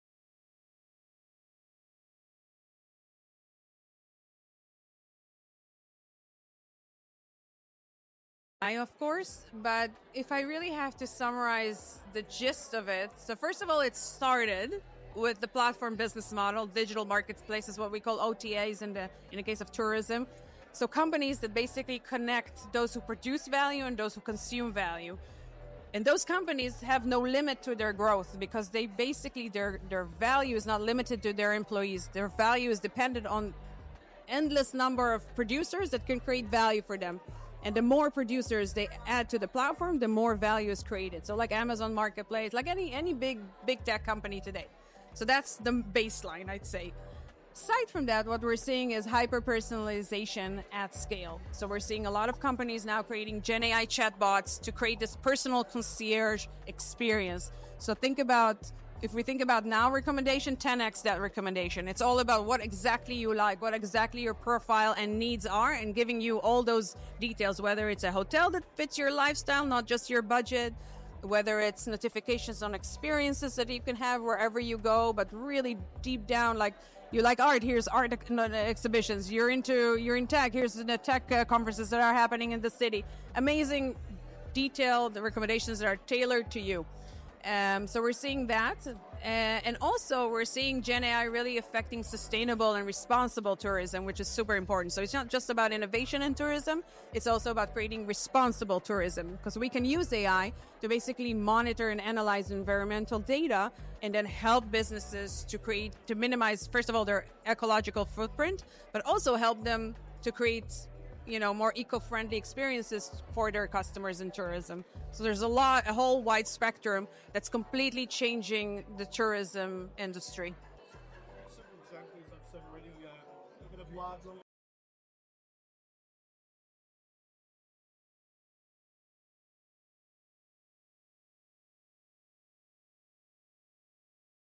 Lital Marom | How Is AI Transforming Tourism Industry
How is AI transforming the Tourism industry?

It began with the rise of platform-based business models that now drive our economy. Recently, Gen AI applications have taken this transformation to a new level.

Just like in many other industries, AI is transforming the way we travel and experience the world. It’s not just about technology—it’s about making our journeys more personal, seamless, and sustainable:

▪️ Personalized Trip Plans
AI can learn from our preferences and past travels to design itineraries that fit us perfectly.

▪️ Simplified Travel Planning
Chatbots and virtual assistants are available 24/7 to help us book trips and solve problems quickly and easily.

▪️ Better Operations Behind the Scenes
AI is also helping travel companies operate more smoothly, from keeping planes in top shape to optimizing prices.

▪️ Breaking Language Barriers
Real-time translation tools make communication easier, helping us connect with new cultures.

▪️ Eco-friendly Travel
AI supports more sustainable travel by offering options that are better for the environment.

AI isn’t just changing the way we book trips—it’s redefining the whole experience of travel. We’re stepping into an era where every journey is more personalized, efficient, and environmentally conscious.

BOOK Lital Marom to speak at your event via the link at the top of this description;